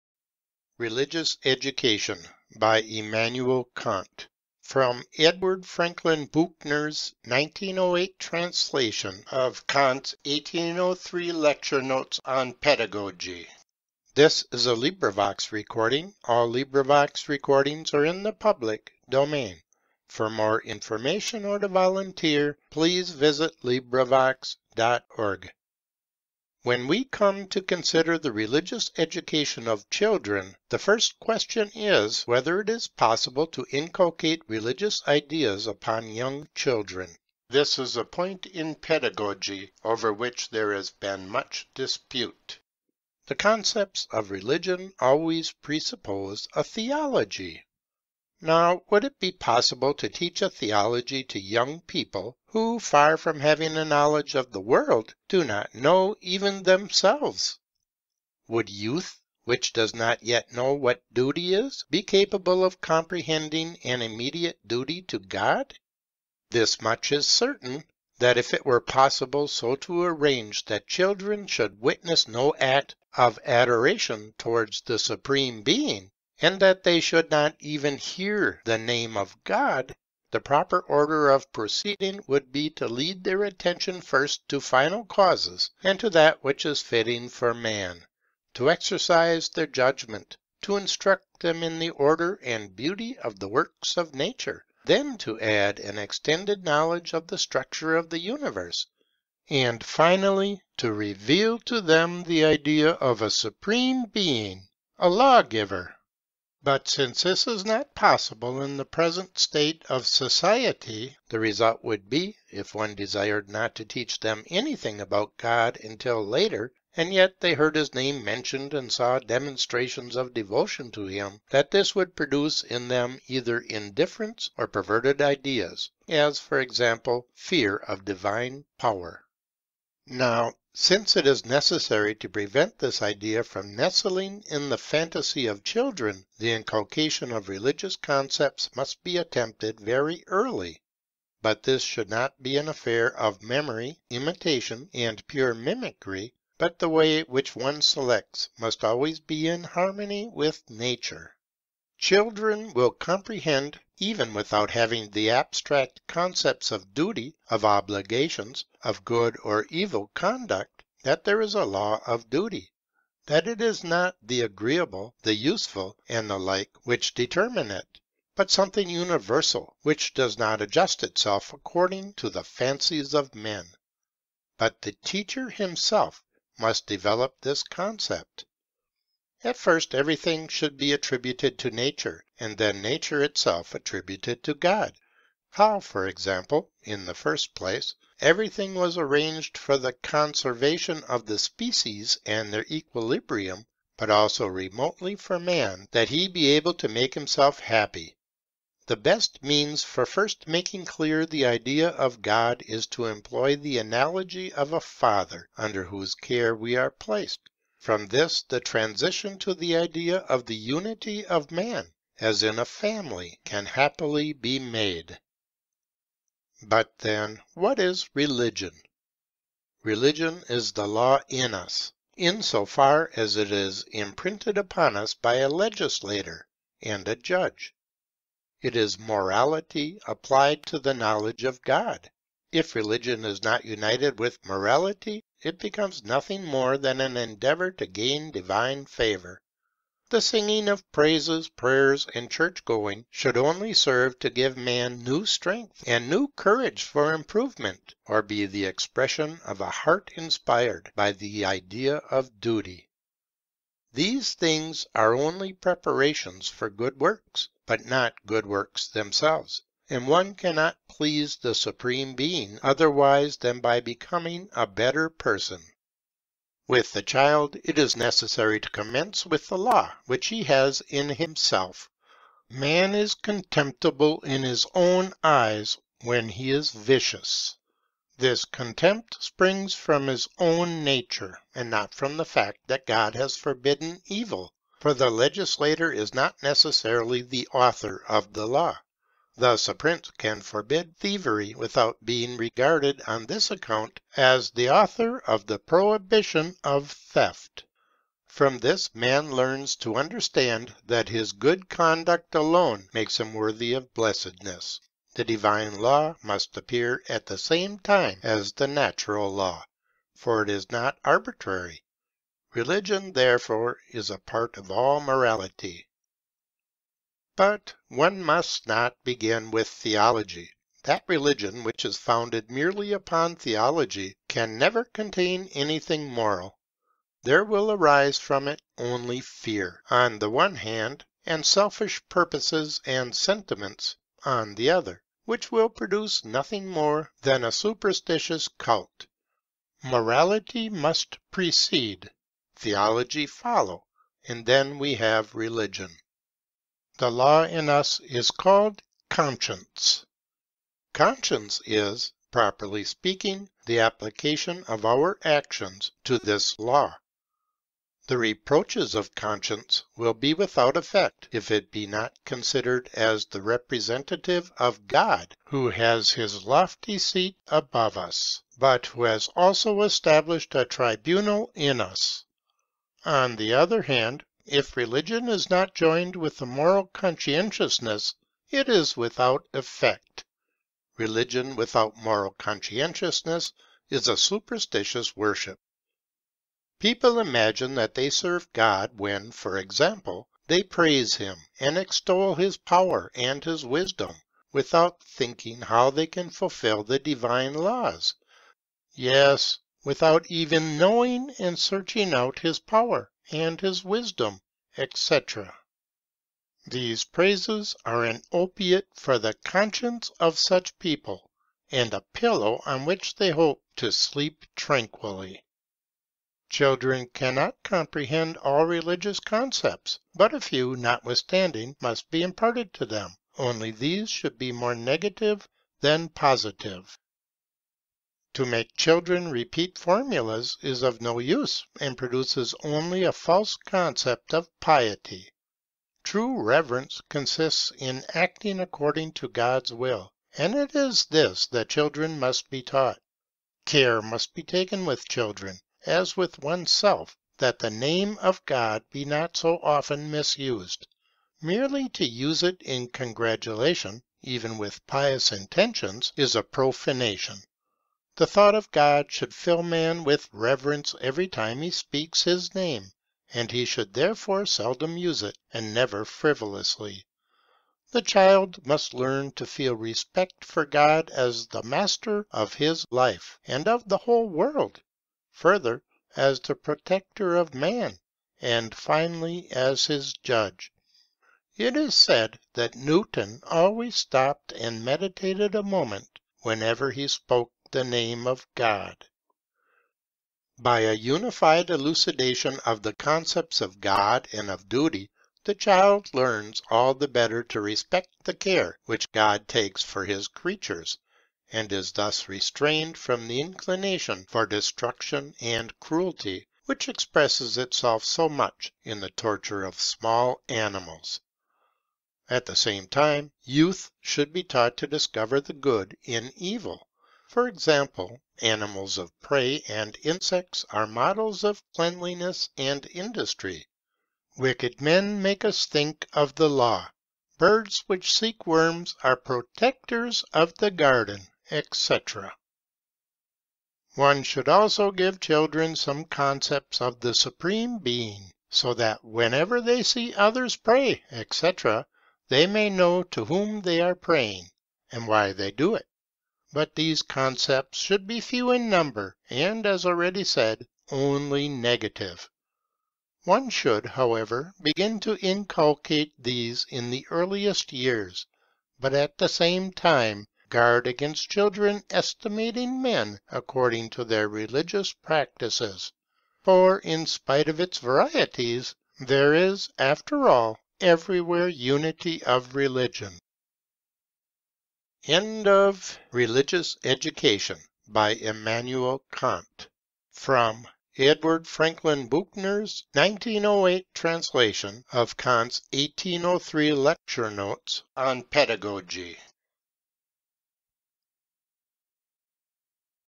Religous Education by Immanuel Kant 1803
Immanuel Kant gave lectures on education in 1809. His notes were translated into English in 1908 as Kan't Lecture notes on Pedagogy. He gave three lectures. 1. Physical Education 2. Moral Education 3. Religious Education. This is a recording of Kant's third lecture.